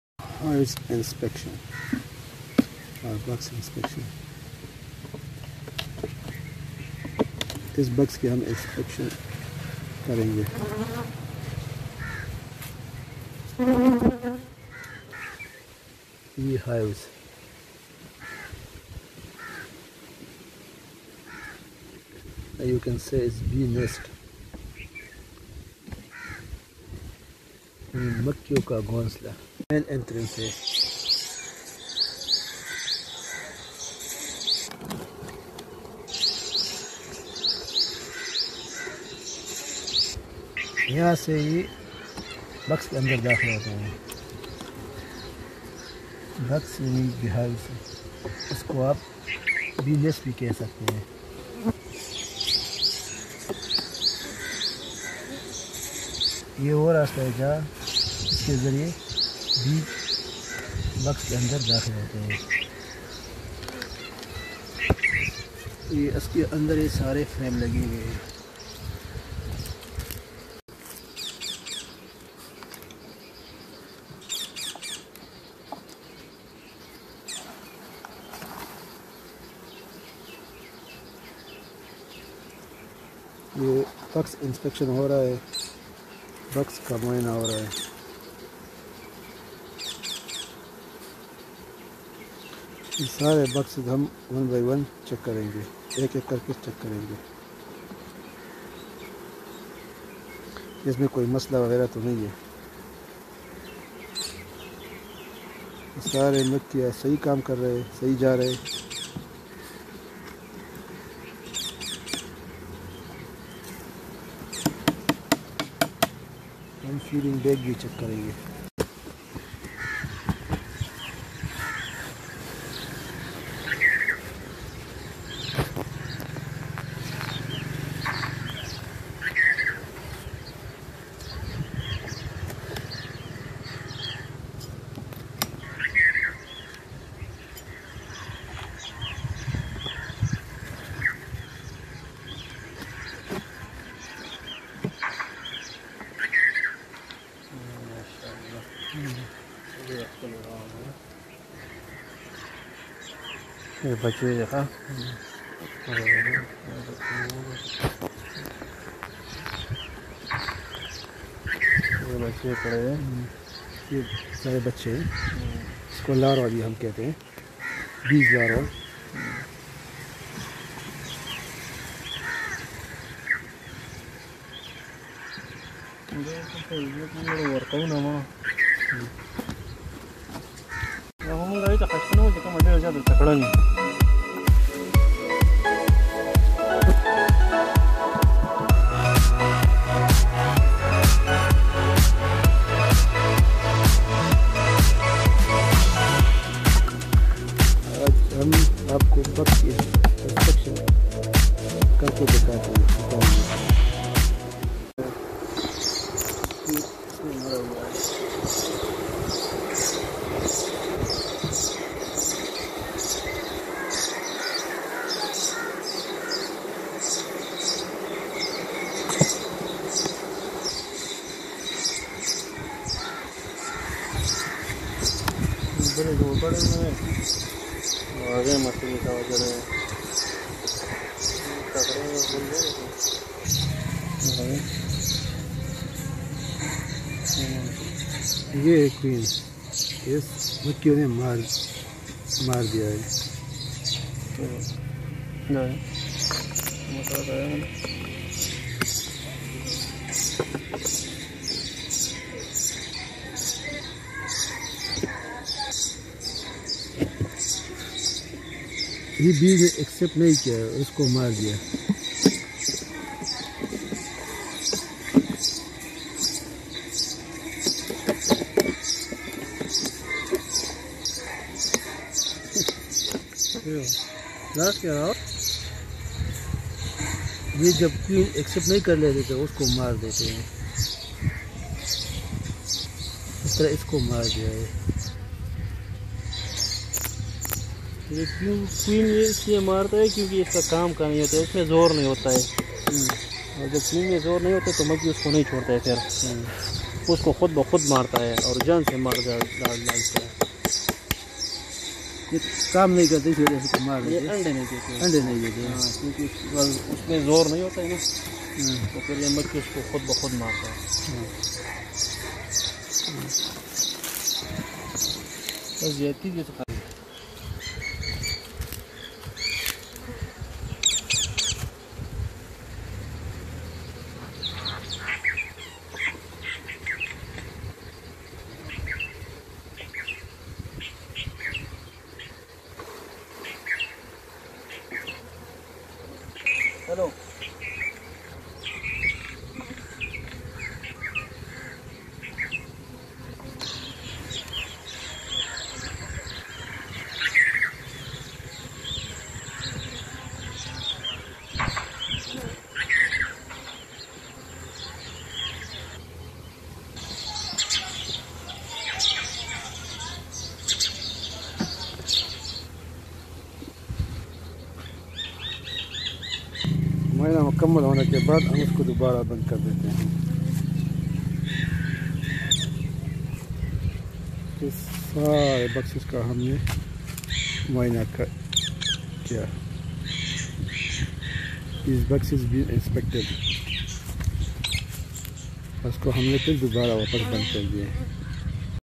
Honey Bee Hive Inspection | Beekeeping - How To Inspect Honey Bee Hive | Honeybee Colony Inspection
Beekeeping, how to inspect bee hives in Hindi and Urdu/Honeybee hives or colony full inspection.